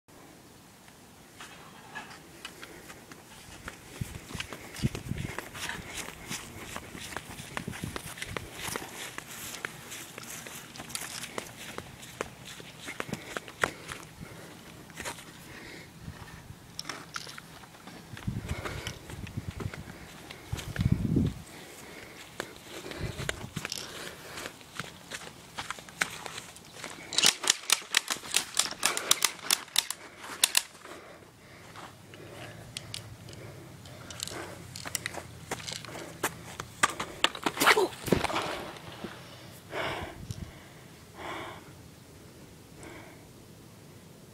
Cod in real life on IPOD 4 (unedited version)
Me and my brother acting like Call of Duty soldiers (video taped with the IPod 4) HD